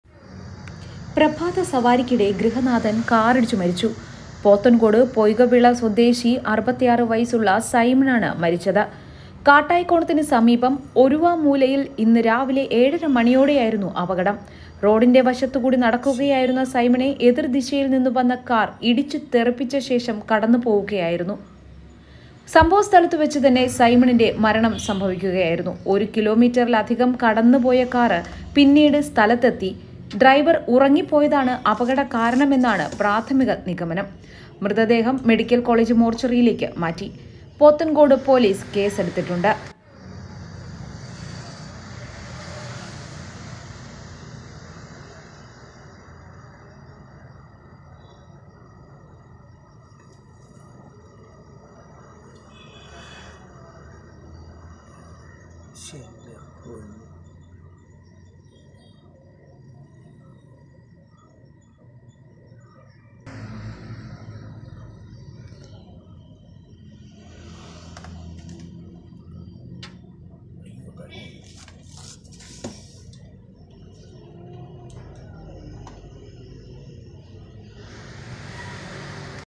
പ്രഭാത സവാരിക്കിടെ കാറിടിച്ച് ഗൃഹനാഥൻ മരിച്ചു | Pothencode accident
പ്രഭാത സവാരിക്കിടെ ഗൃഹനാഥൻ കാറിടിച്ചു മരിച്ചു. പോത്തൻകോട് പൊയ്കവിള സ്വദേശി സൈമൺ (66) ആണ് മരിച്ചത്. കാട്ടായിക്കോണത്തിനു സമീപം ഒരുവാമൂലയിൽ ഇന്ന് രാവിലെ ഏഴര മണിയോടെയായിരുന്നു അപകടം.